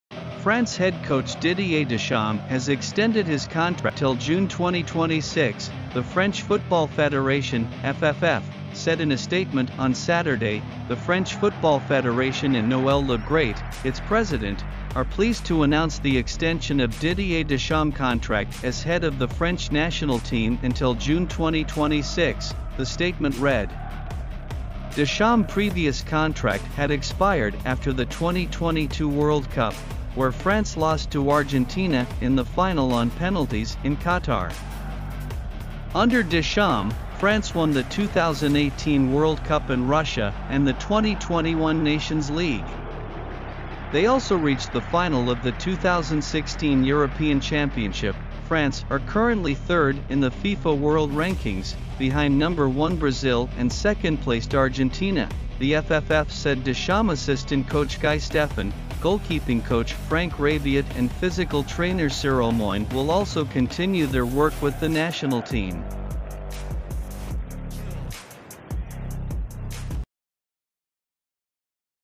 France head coach Deschamps extends contract until 2026
France head coach Deschamps extends contract until 2026.Football,FIFA,official,Soccer,Futbol,Futebol,Fußball,Fussball,Calcio,Voetbal,